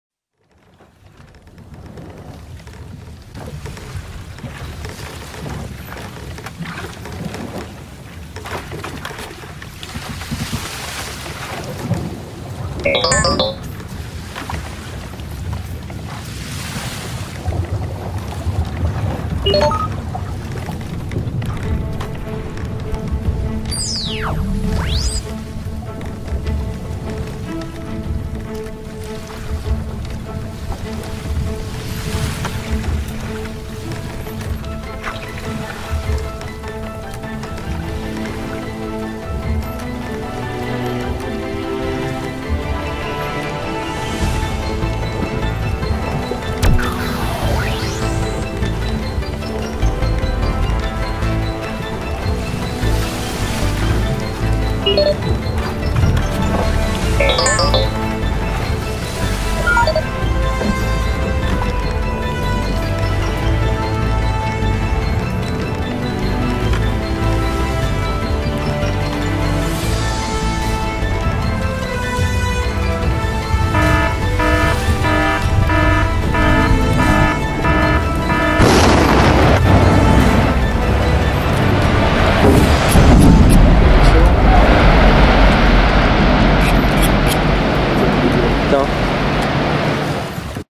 PROBANDO TORPEDO POSEIDON INSTALADO EN CATAMARÁN
Hoy en día hay que llevar de todo en el barco porque el mundo está loco...